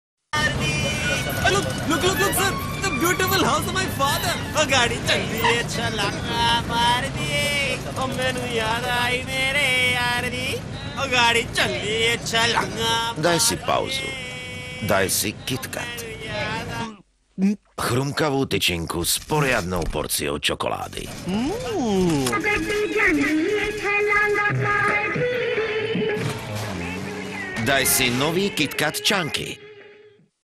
Kit Kat Chunky - Taxi Driver (2002, Croatia)
An overjoyed taxi driver compels his passenger to stuff a yummy snack into his gob, whether or not that actually gives him a break. There is also a version of this ad made in India itself.